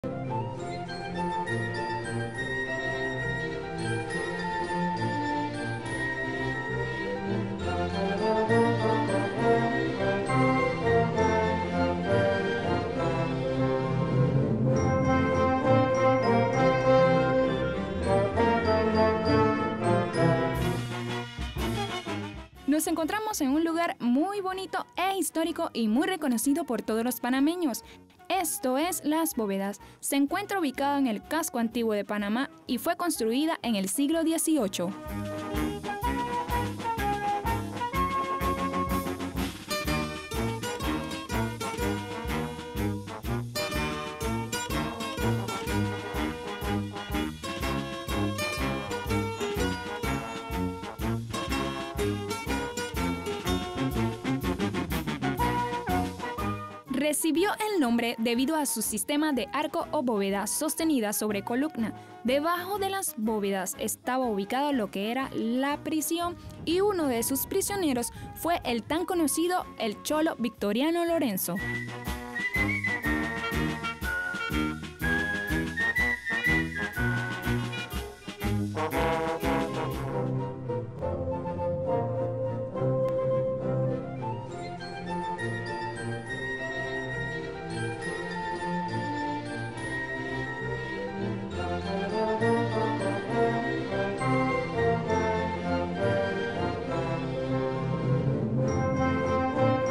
Las Bóvedas - Panamá
Las Bóvedas es el nombre como comúnmente se le conoce al conjunto de obras que forman las murallas que rodean la Plaza de Francia en la ciudad de Panamá.1 El conjunto monumental consta de siete espacios abovedados que conformaban el sistema defensivo de la ciudad. El monumento se encuentra en el Casco Antiguo de Panamá.